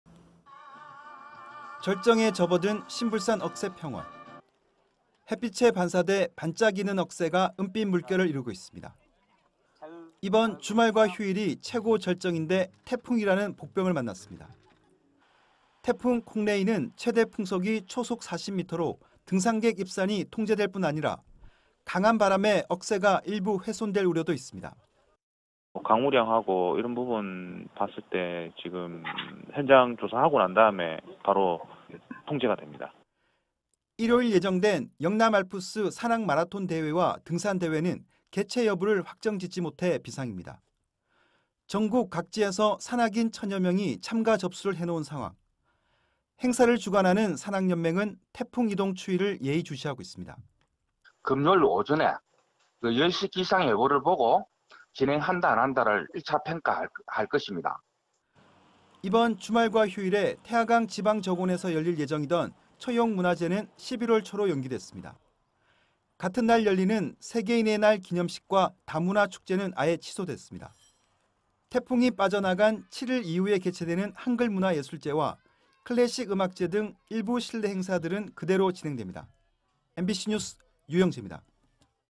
20181004 야속한 태풍..축제 줄줄이 차질
가을 성수기를 맞아 다양한 축제가 열릴 예정이었지만
제25호 태풍 '콩레이'가 북상하면서 축제가 취소되거나
연기되는 등 줄줄이 차질을 빚고 있습니다. 🌪

울산MBC는 여러분의 소중한 제보를 기다립니다.
울산MBC 카카오톡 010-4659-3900